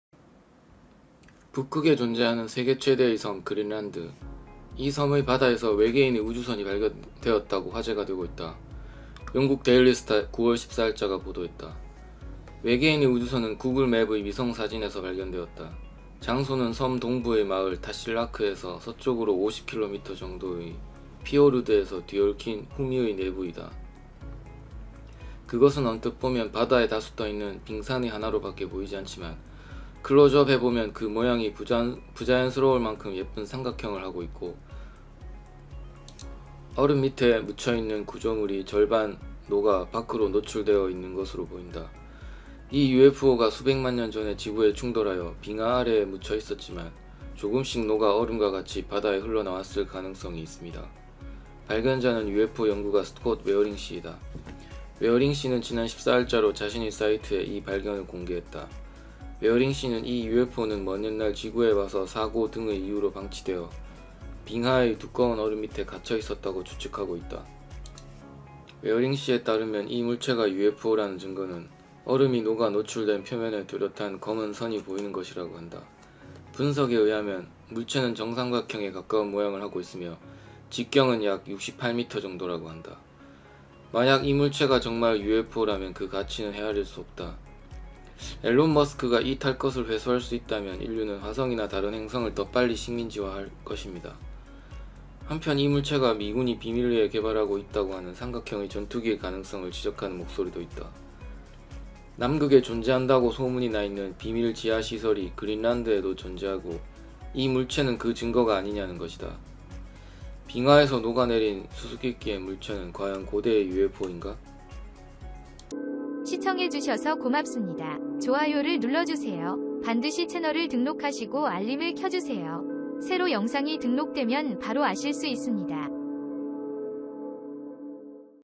북극 빙하 아래에서 "고대의 UFO”가 구글어스로 발견되다! 좌표도 판명, 얼음이 녹아 구조물이 노출!
북극에 존재하는 세계 최대의 섬 그린란드. 이 섬의 바다에서 「외계인의 우주선」이 발견되었다고 화제가 되고있다.

본 채널의 프리미엄 회원으로 가입하여 회원전용 영상들을 보세요.